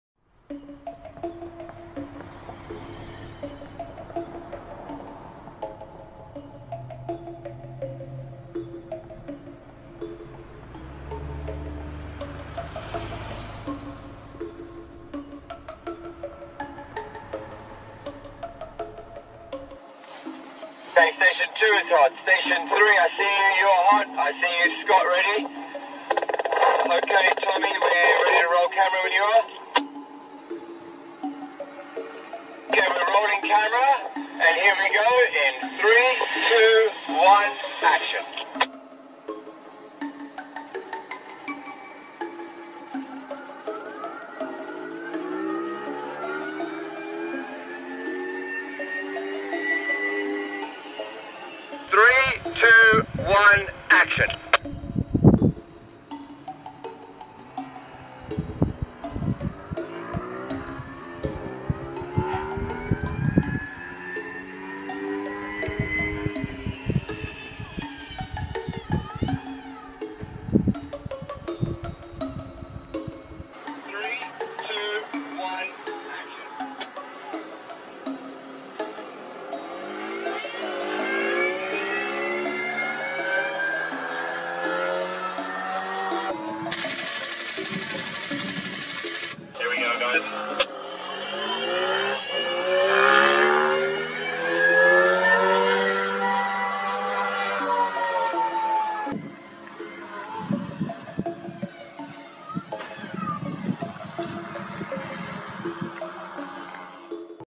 Tom Cruise performs insane BMX stunt for new movie Mission Impossible 7 in UK
Worldwide superstar Tom Cruise performs an insane BMX stunt for new movie in long running franchise Mission Impossible 7 in Oxfordshire in the UK.

My Top Performing Video so Far.